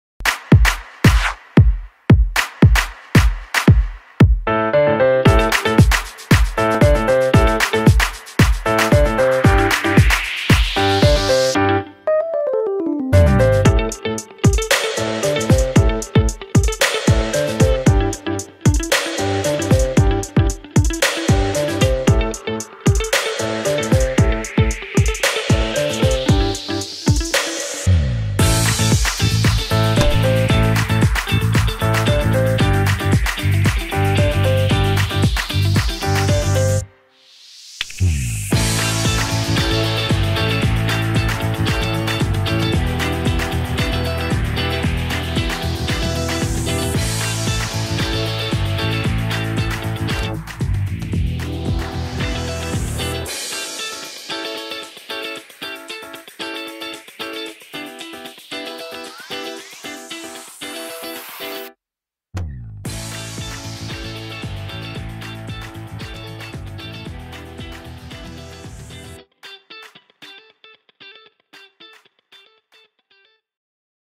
Samsung Galaxy Chromebook 2 Unboxing || Official Unboxing 2021
The Samsung Galaxy Chromebook 2, here with brilliant features including QLED-enabled immersive viewing.